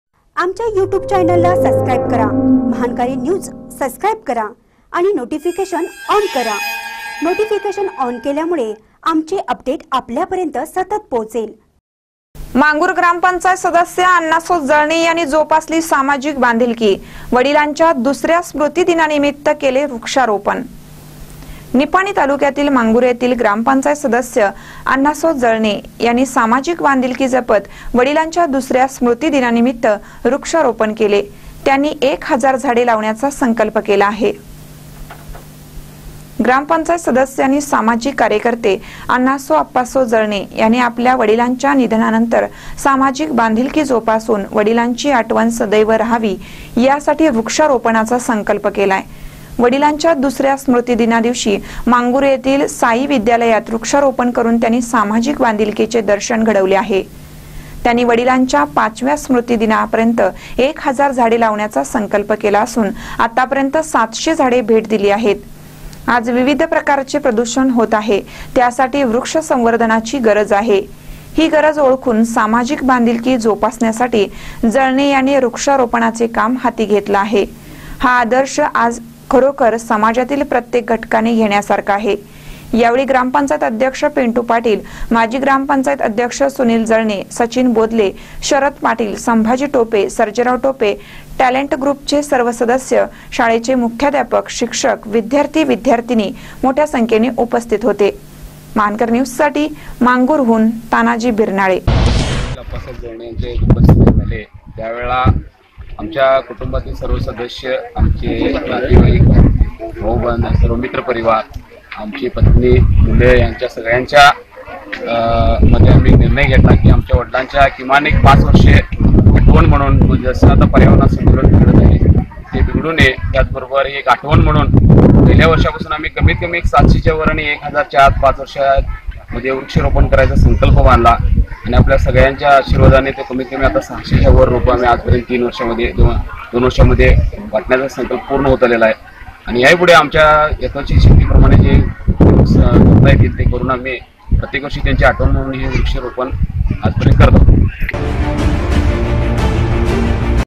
मांगूर ग्रा.पं. सदस्य आण्णासो जळणे यांनी जोपासली सामाजिक बांधिलकी
मांगूर ग्रा.पं. सदस्य आण्णासो जळणे यांनी जोपासली सामाजिक बांधिलकी :  वडिलांच्या दुस-या स्मृतीदिनानिमित्त केले वृक्षारोपण